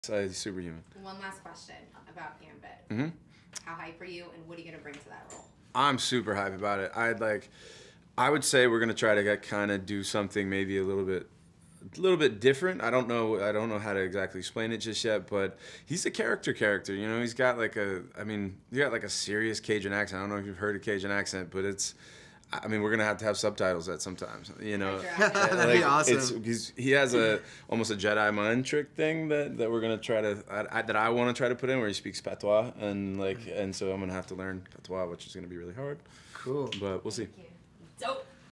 Channing Tatum - On Playing Gambit
Channing Tatum talks to Latino-Review about what he would like to bring the character Gambit in the upcoming X-Men film.